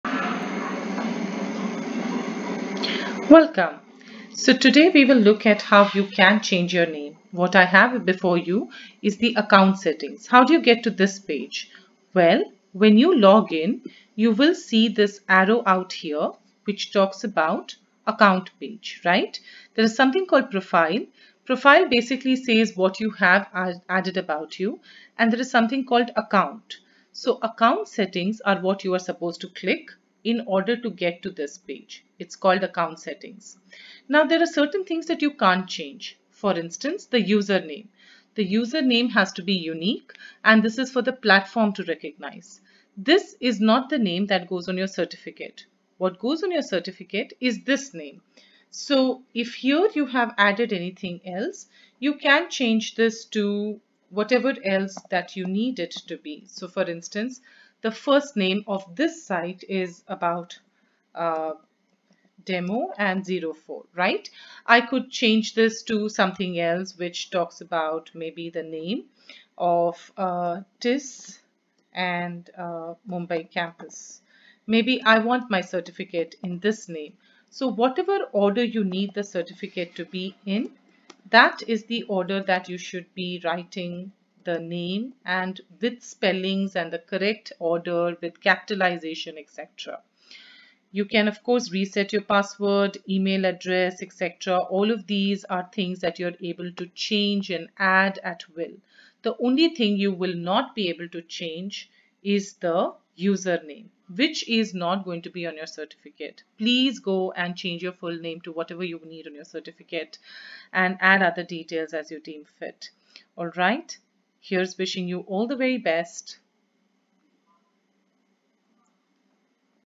[TISSx | Help Guide] Editing Your Name - Account Settings
This video showcases how you can edit your name and other details after registering on TISSx. Everything but your username, which is unique as per system requirements can be changed.

It is important to ensure that your name is the exact same as you want in your certificate.

Happy Learning!